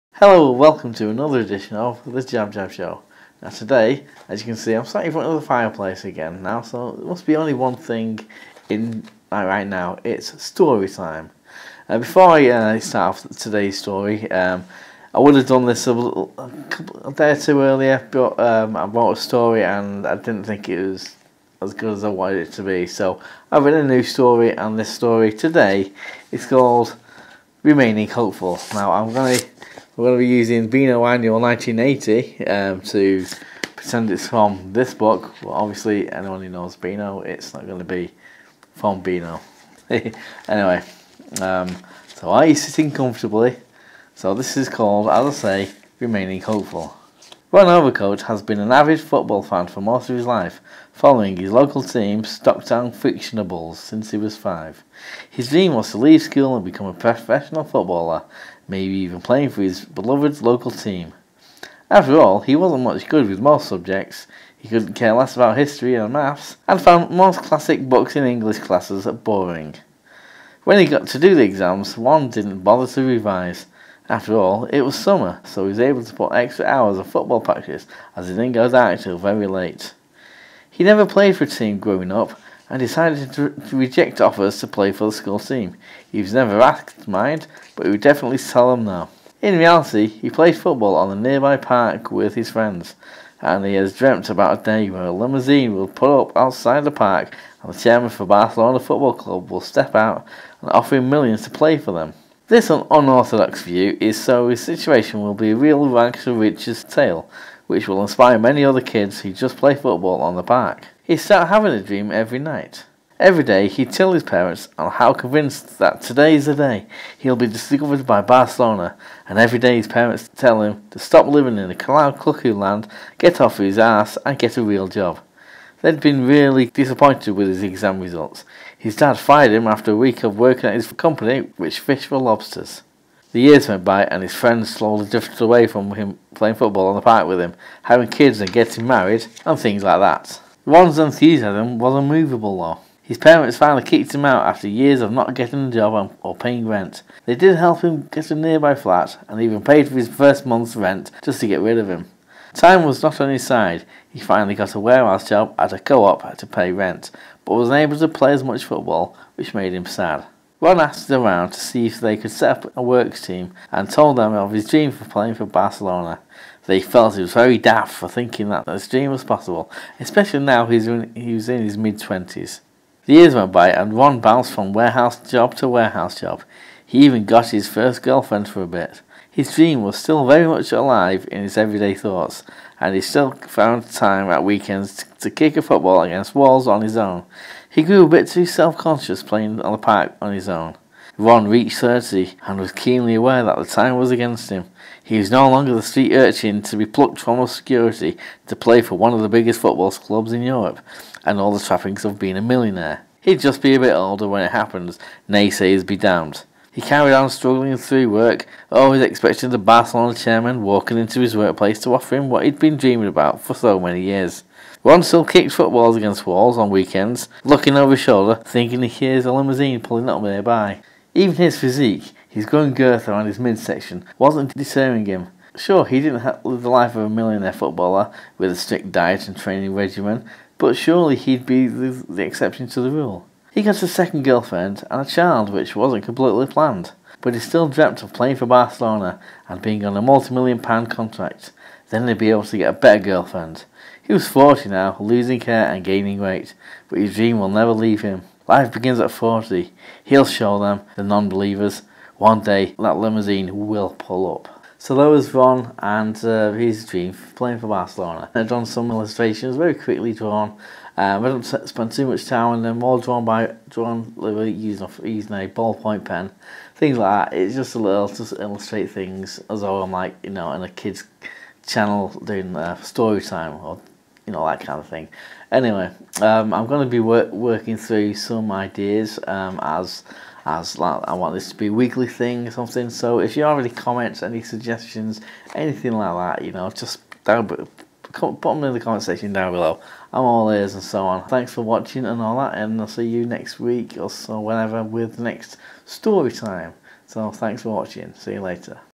It's Story Time! Remaining Hopeful
Hey everyone!
Here's the sceond in an ongoing series of Jackanory-style video that I originally uploaded onto my other YouTube channel The JabJab Show on 03/05/20.
I wrote the short story and drew the accompanying illustrations.